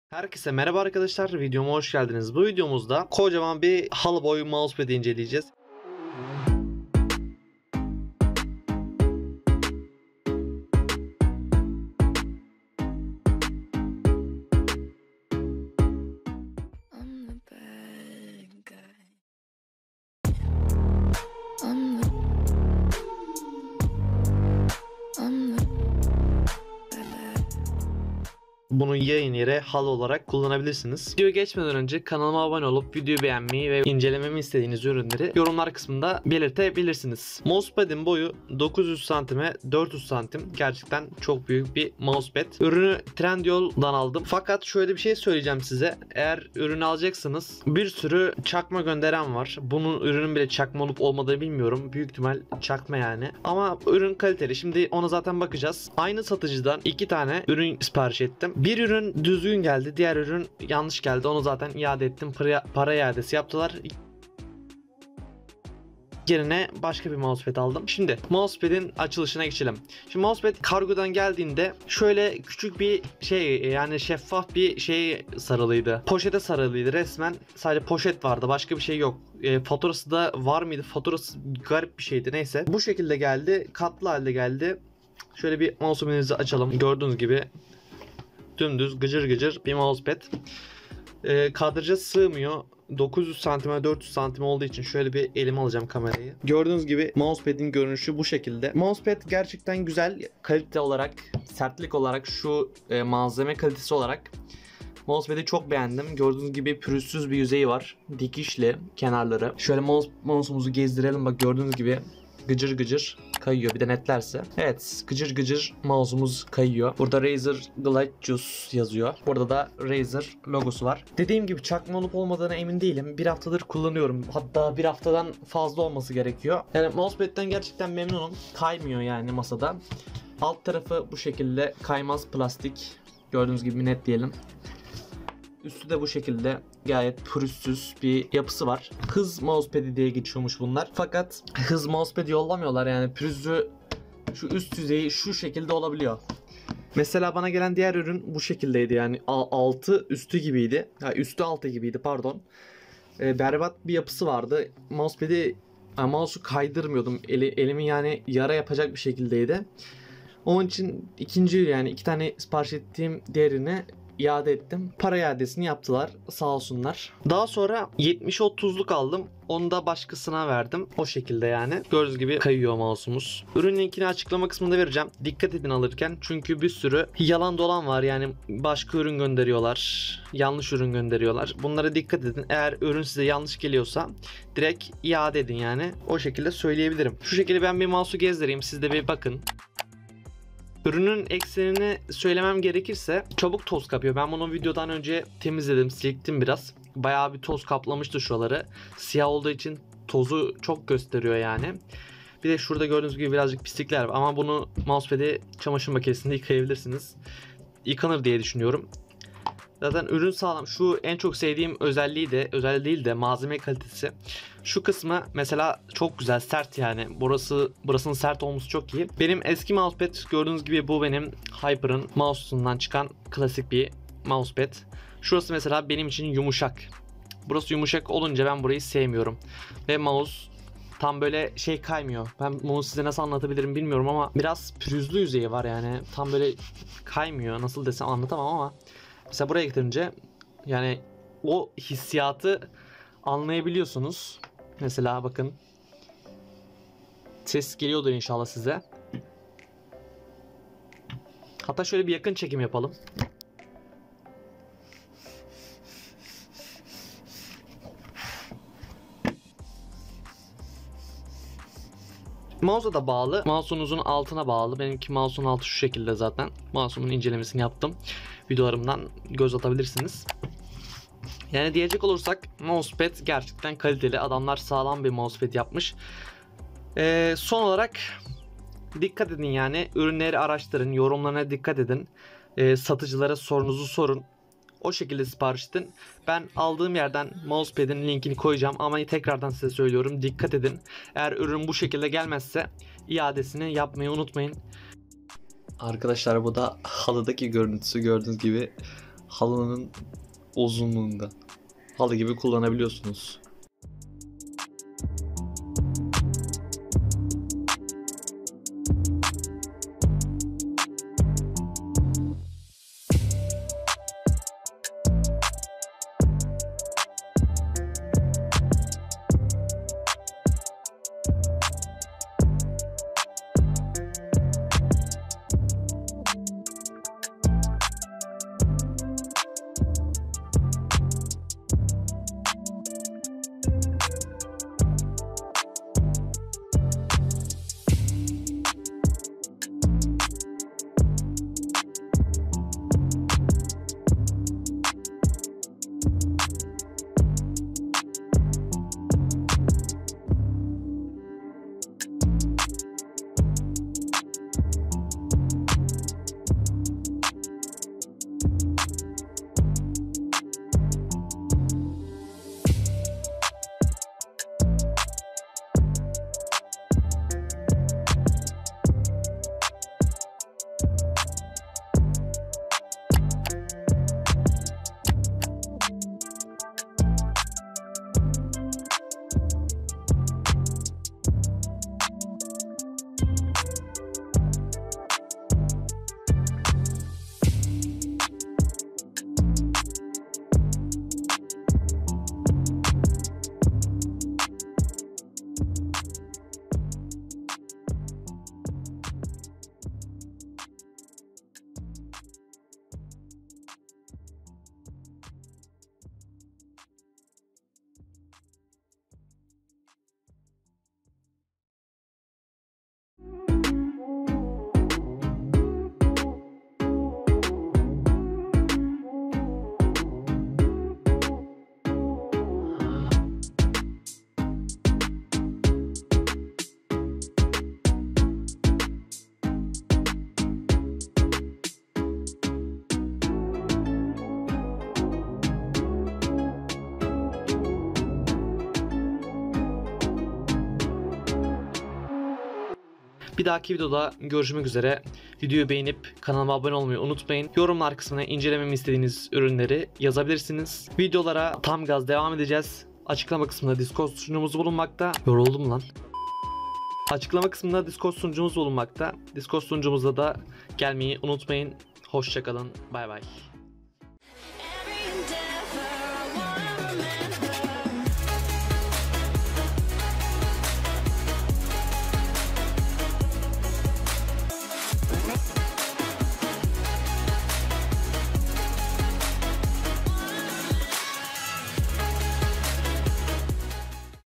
90x40 HALI BOY MOUSEPAD İNCELEMESİ! (Razer Galiathus Mousepad)
Razer Goliathus 90x40 mousepad inceledim. Halı boy mousepad süper!

👇Kesinlike izlemeniz gereken video albümlerim!